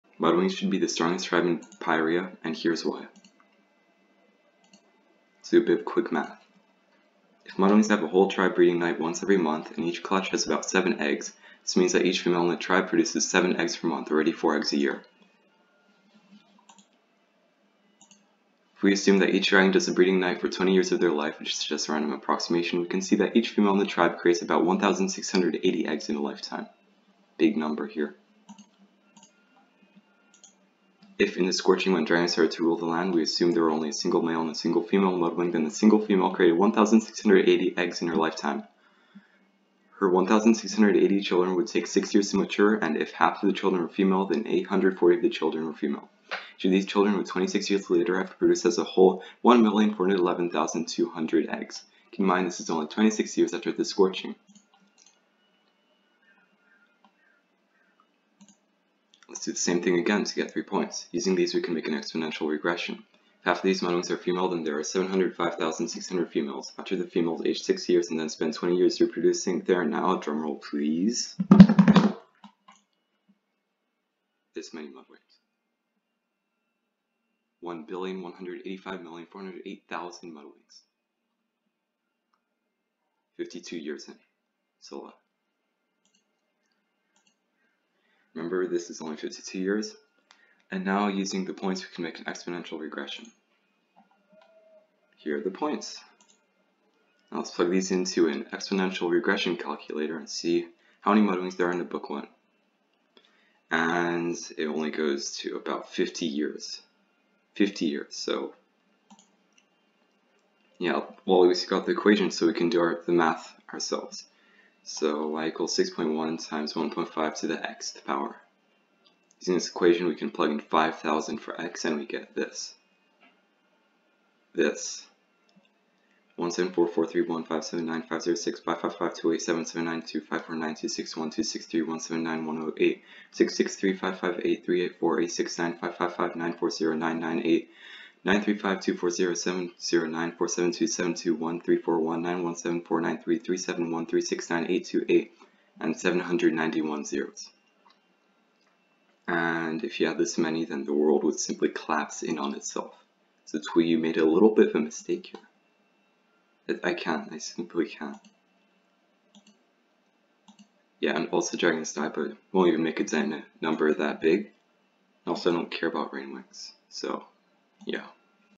How Many MudWings are there actually? WOF Explained #1 (The Deepest Mud Puddle Ever.)
The math behind why there are more MudWings than there are stars in the sky, Humans on the Earth, and square miles in the world - combined.